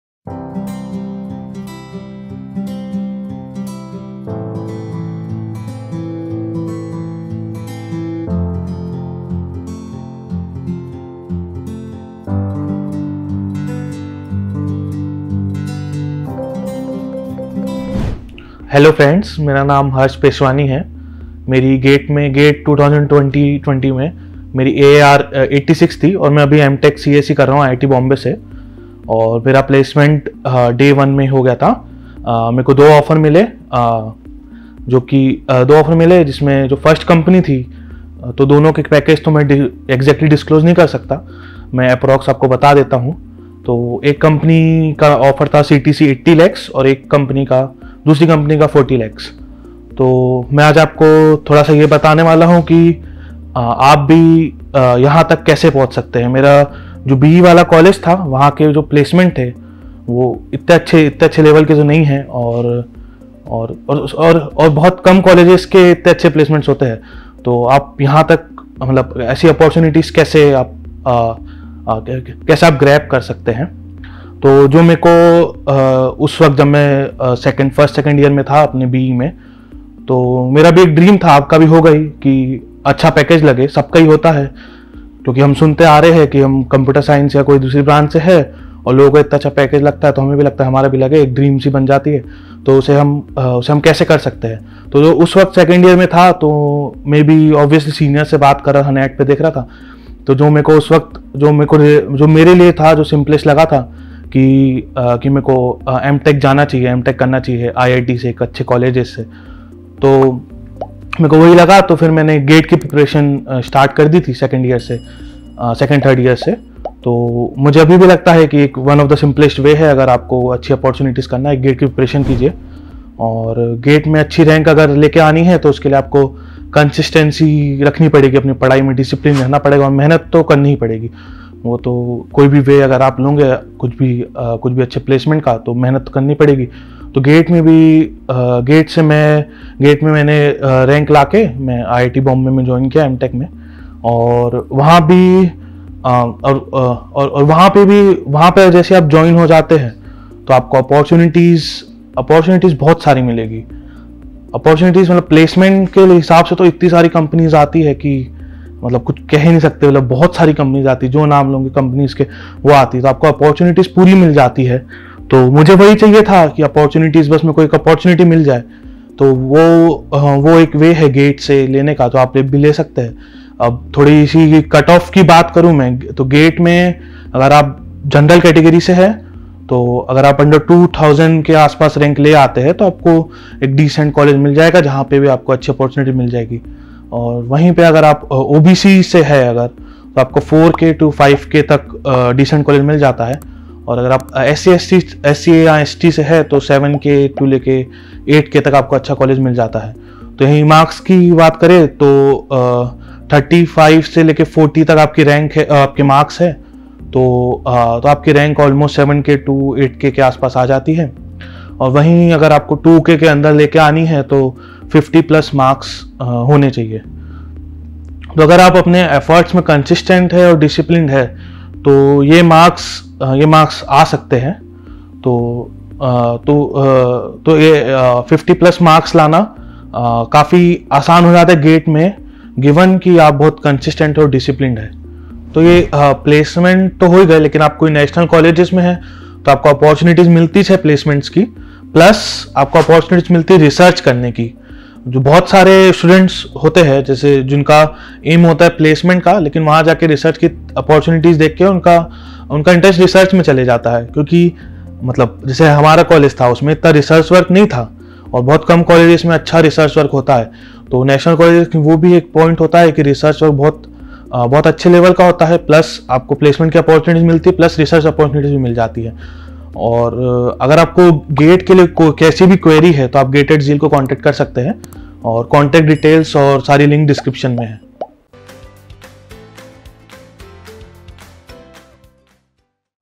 How to grab 1 crore placement | IIT Bombay Placements | Gate at Zeal
Welcome you all to GATE at ZEAL, as you are all aware that we frequently share the video of GATE achievers to boost your confidence and to provide you guidance.

So today we have Mr. Harsh Peshwani GATE AIR 86th rank holder, today he will share his experience and success story with you all, and also he will let you know about the benefits of having good points in the GATE examination.

So dear students watch the video carefully and learn from it.

Watch the video as before start studying it is very important for you all to know the syllabus.

⏩ Gate CS Syllabus Videos